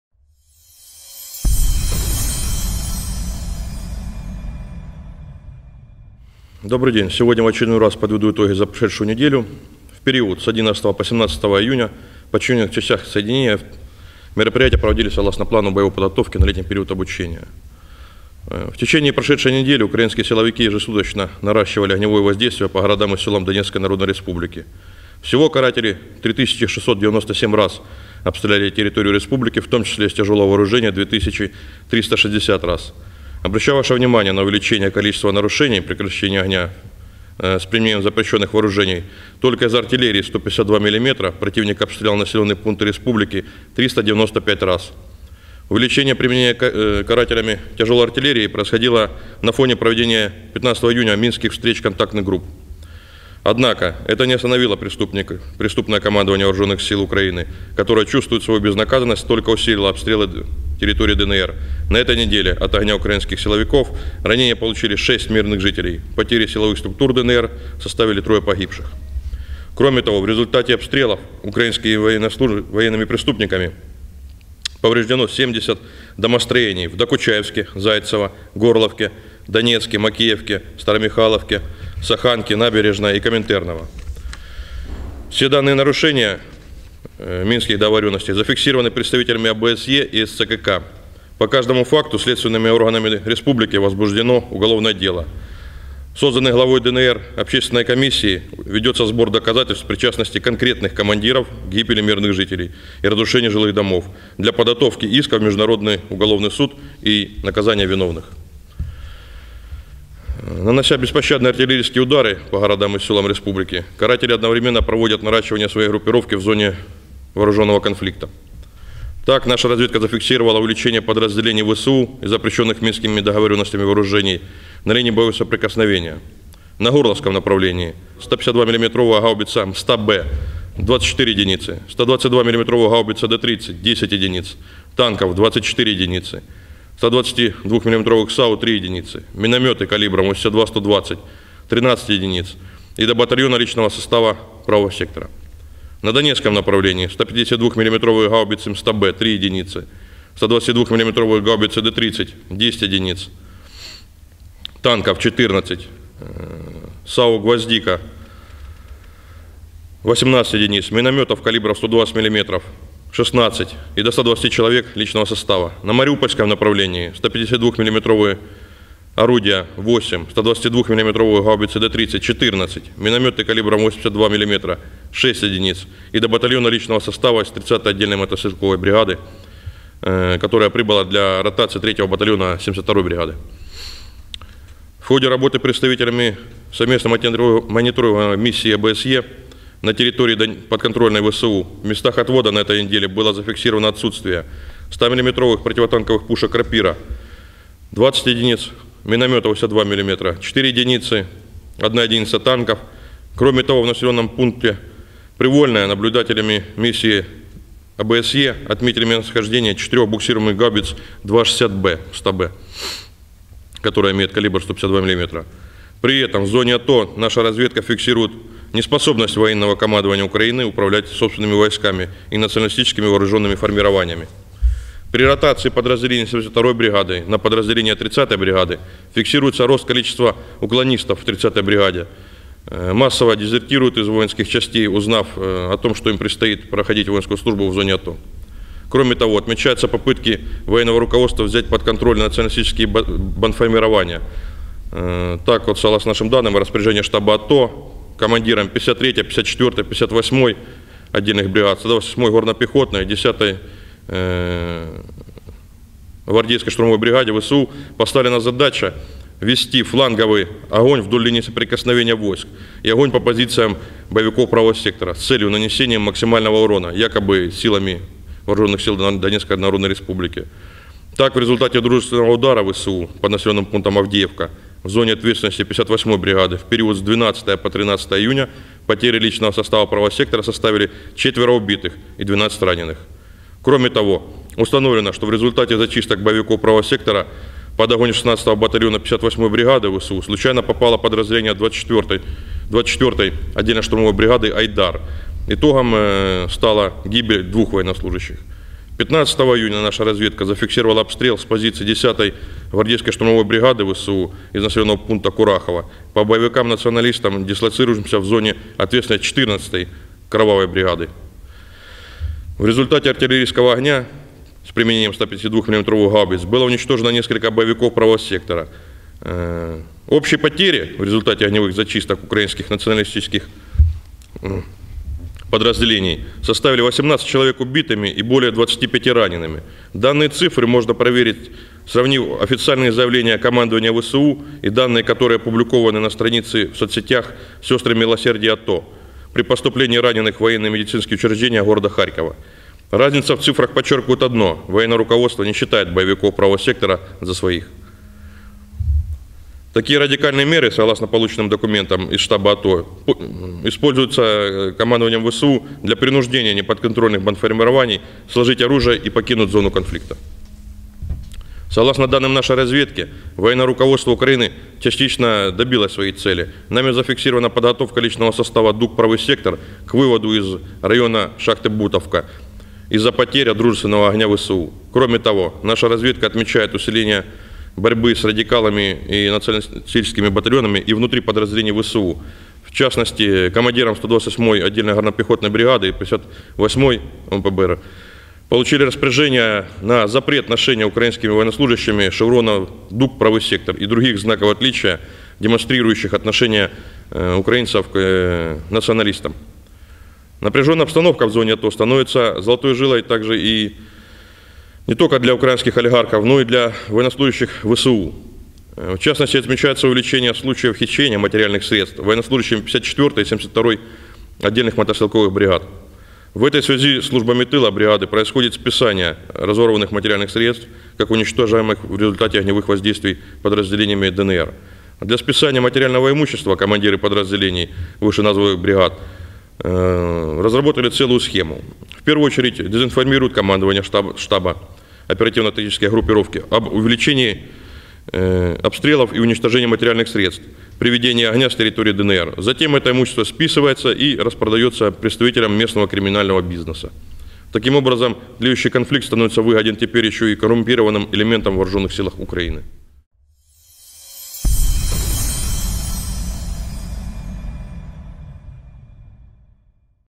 ВСУ обстреляли территорию ДНР свыше 3,5 тысяч раз
Увеличение применения ВСУ тяжелой артиллерии происходило на фоне проведения 15 июня минских встреч контактных групп. Однако, это не остановило украинское командование. На этой неделе от огня ВСУ ранения получили 6 мирных жителей, потери силовых структур ДНР составили трое погибших.

Общественная комиссия, созданные главой ДНР, ведет сбор доказательств причастности конкретных командиров к гибели мирных жителей и разрушению жилых домов для подготовки исков в международный уголовный суд и наказанию виновных. Разведка ДНР продолжает фиксировать увеличения подразделений ВСУ и запрещенных минскими договоренностями вооружений на линии боевого соприкосновения.